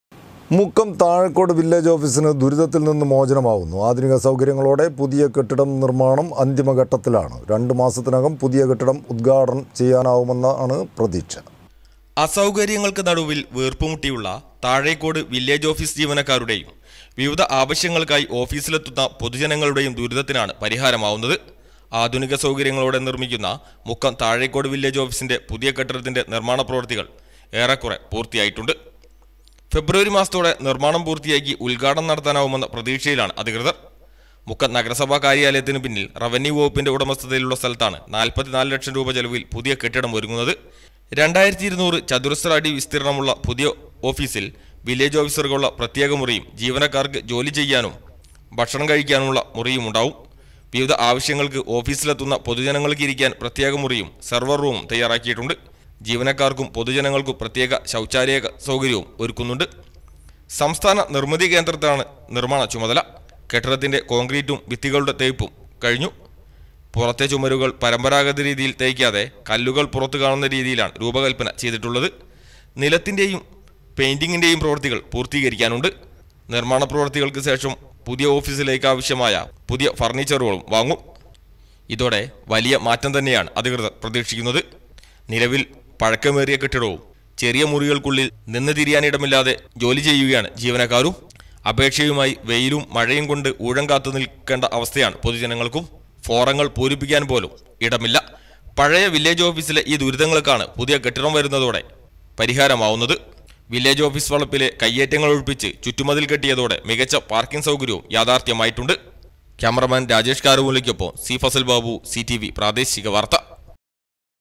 മുക്കം താഴെക്കോട്- വില്ലേജ് ഓഫീസിന് ദുരിതത്തിൽ നിന്ന് മോചനമാവുന്നു.2 മാസത്തിനകം പുതിയ കെട്ടിടം
മുക്കം താഴെക്കോട് വില്ലേജ് ഓഫീസിന് ദുരിതത്തിൽ നിന്ന് മോചനമാവുന്നു. ആധുനിക സൗകര്യങ്ങളോടെ പുതിയ കെട്ടിടം നിർമ്മാണം അന്തിമഘട്ടത്തിലാണ്. 2 മാസത്തിനകം പുതിയ കെട്ടിടം ഉദ്ഘാടനം ചെയ്യാനാവുമെന്നാണ് പ്രതീക്ഷ